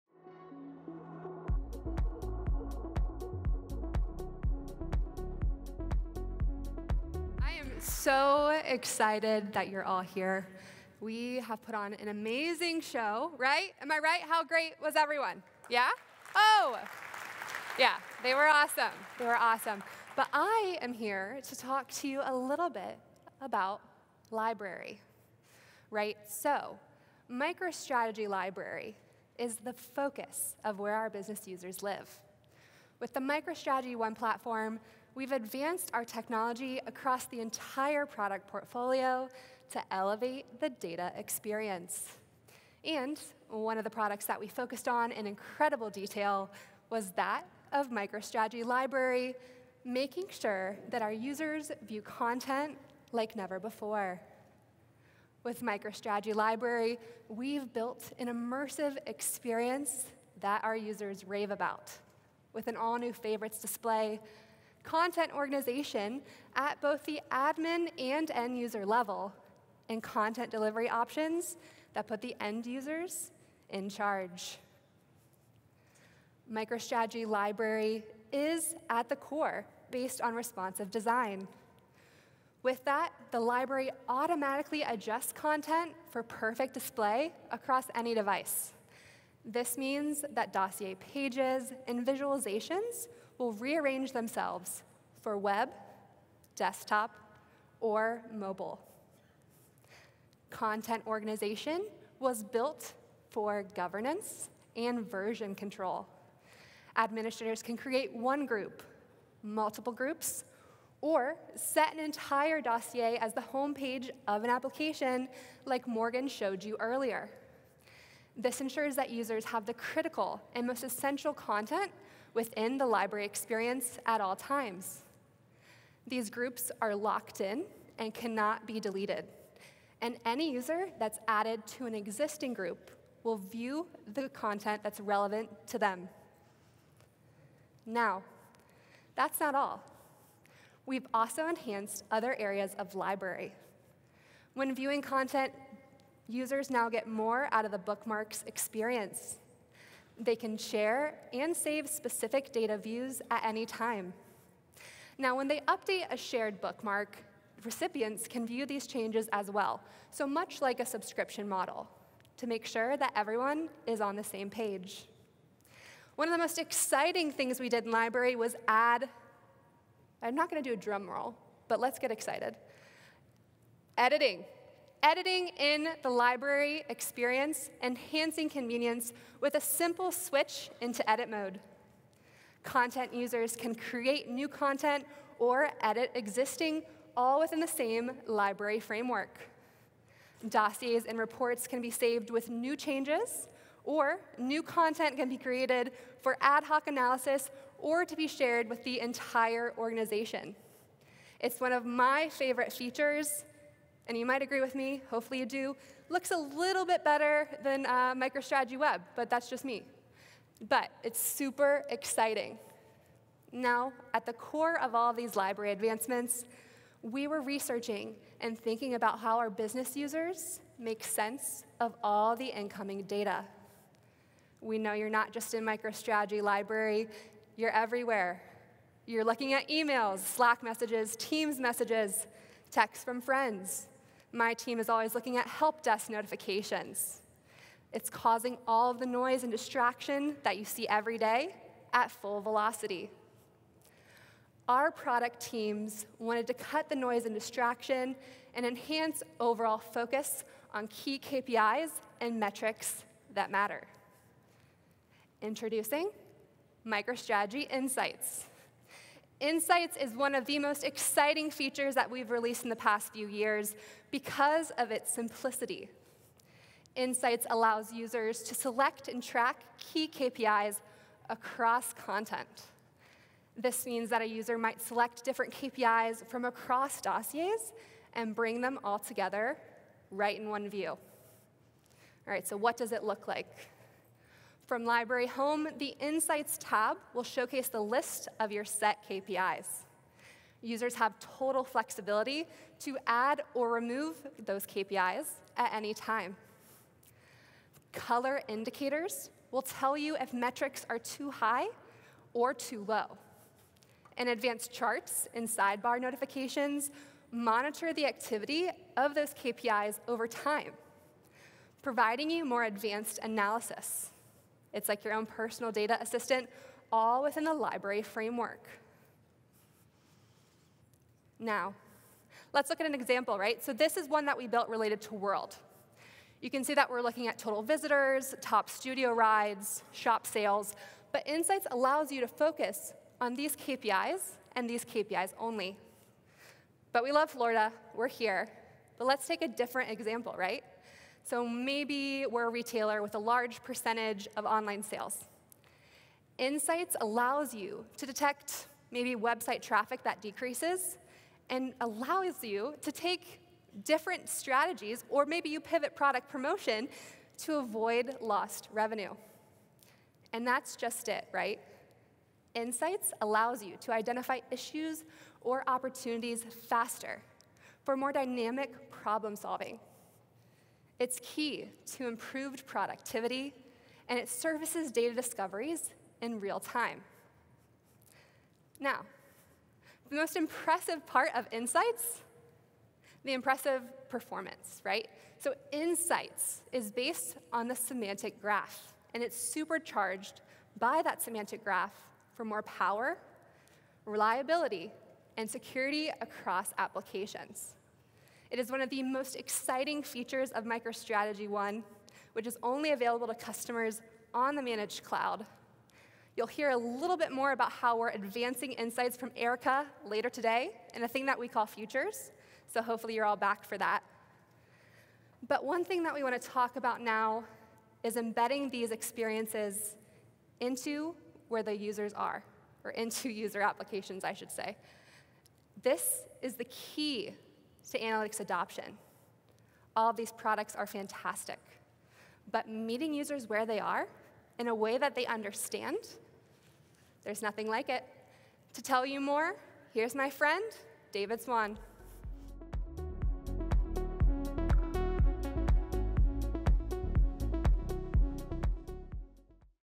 Enhanced Data Discovery with Library
Product expert, Holly Stevens, reveals how the development of the MicroStrategy ONE platform has led to an entirely revamped Library experience. Library is an immersive experience that elevates the data experience and empowers organizations to create personalized applications. Plus, you'll learn about MicroStrategy Insights, the data alerting feature supercharged by the Semantic Graph.

SPEAKER:
Holly Stevens - Senior Product Marketing Manager, MicroStrategy